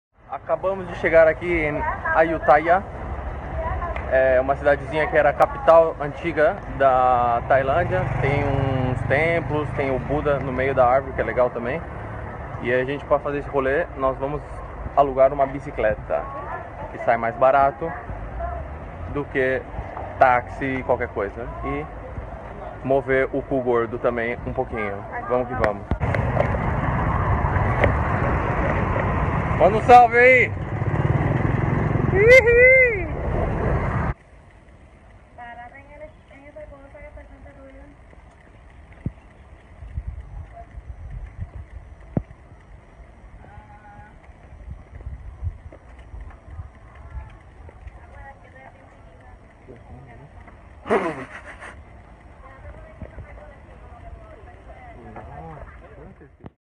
Melhor dica pra conhecer Ayuthaya BARATO
🇺🇸 If you are thinking to go to Ayutthaya, do not get taxi, bus or tuk tuk. Best option is rent a bike. Cheap and lovely views.
🇪🇸 Si estas pensando en ir a Ayutthaya olvidate del taxi, autobus o tuk tuk, la mejor opcion es rentar una bici, mas barato y vistas encantadoras.
🇧🇷 Se voce estiver pensando em ir a Ayutthaya esqueçe taxi, onibus ou tuk tuk. A melhor opção é alugar uma bicicleta. Barato e vistas lindas.